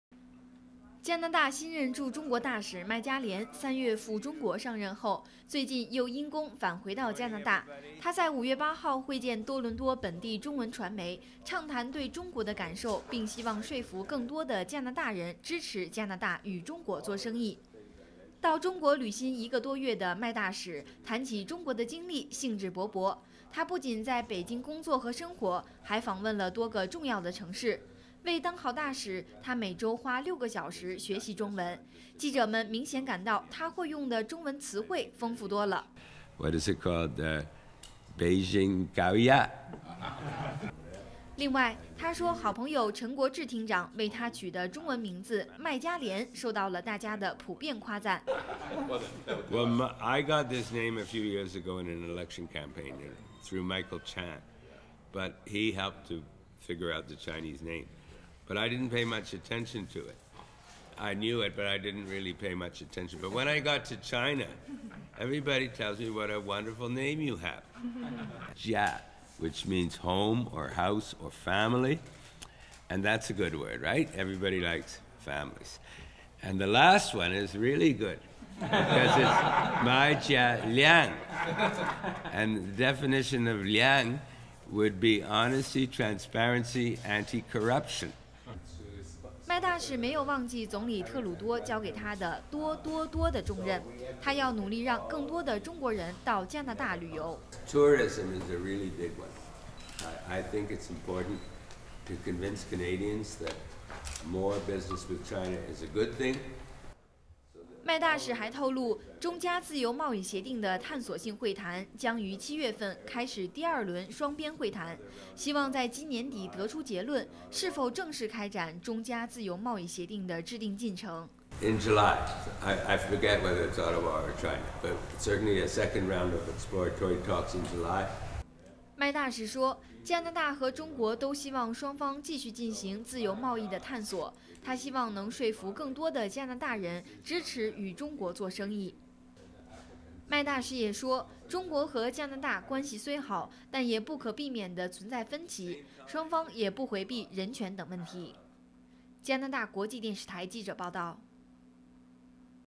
麦家廉：中国人都夸我的中文名超赞
加拿大新任驻中国大使麦家廉三月赴中国上任后，最近又因公返回到加拿大。他在5月8日会见多伦多本地中文传媒，畅谈对中国的感受并希望说服更多的加拿大人支持加拿大与中国做生意。